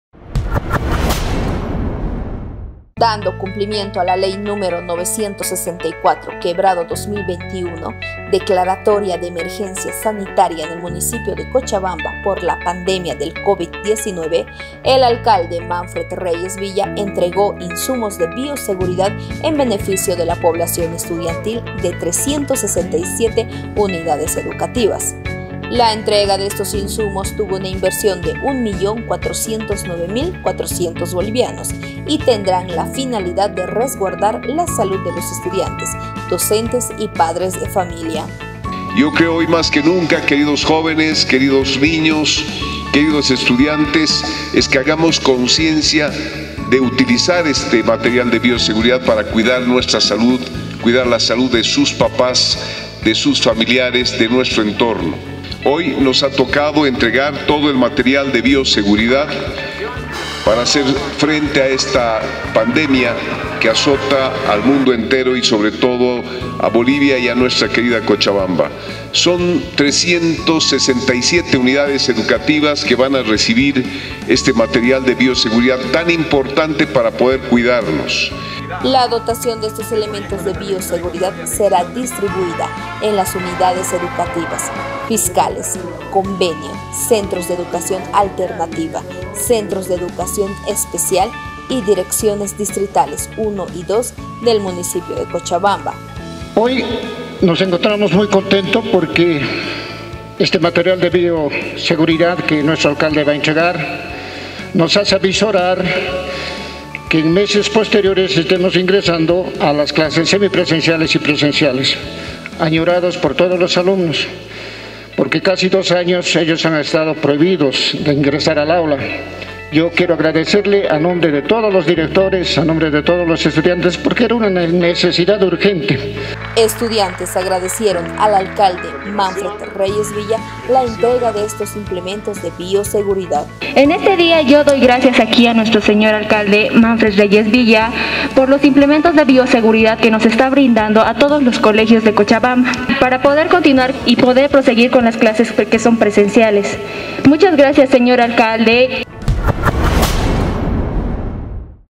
ALCALDE MANFRED REYES VILLA ENTREGÓ INSUMOS DE BIOSEGURIDAD EN BENEFICIO DE LA POBLACIÓN ESTUDIANTIL
Dando cumplimiento a la Ley 964/2021 “Declaratoria de Emergencia Sanitaria en el municipio de Cochabamba por la pandemia del Covid-19”, el alcalde Manfred Reyes Villa entregó insumos de bioseguridad en beneficio de la población estudiantil de 367 Unidades Educativas.